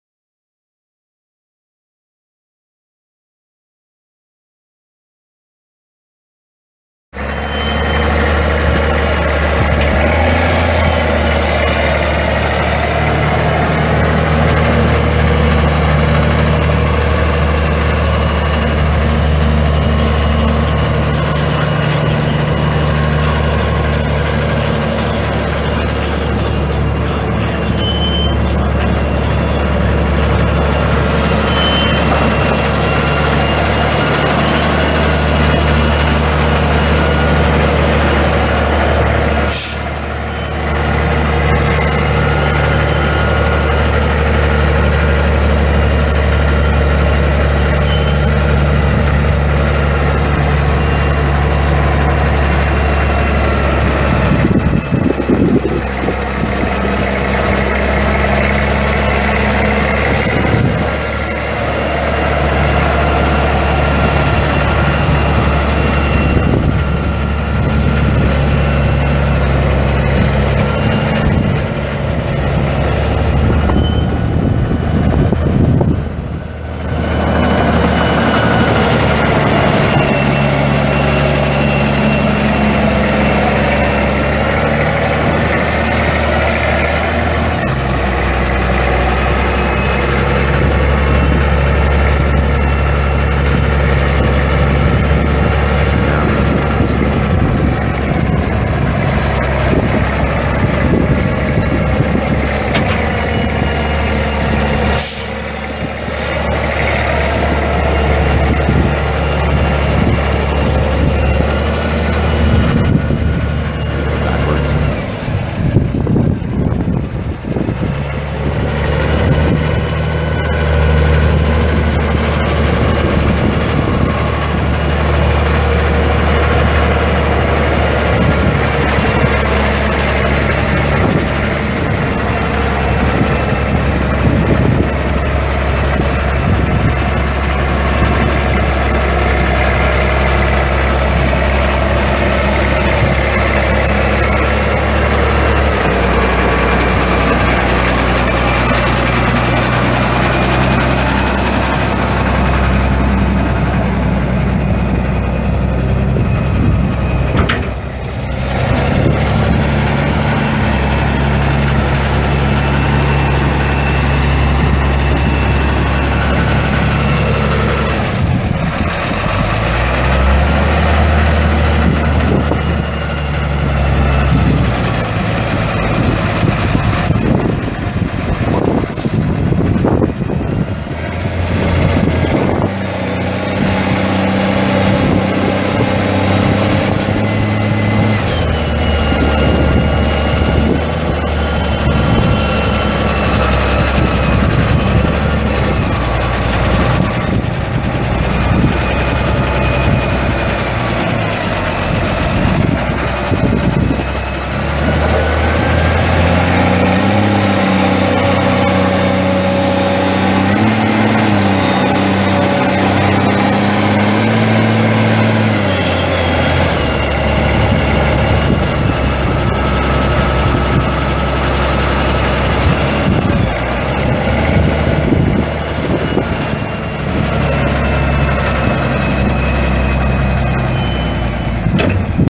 1978 Caterpillar 966C wheel loader for sale | no-reserve Internet auction April 13, 2017
View a detailed listing of this item and place bids

1978 Caterpillar 966C wheel loader
4,563 hours on meter
Caterpillar 3306 six cylinder turbo diesel engine
170 HP
Powershift transmission
4F-4R gears
EROPS
Heat
Suspension seat
Wipers
Air dryer
Front and rear work lights
111"W general purpose bucket
Bolt-on and weld-on cutting edge
23.5-25 tires
Serial 76J12247
It's located in Newton, IA, and we're selling it to the highest bidder regardless of price.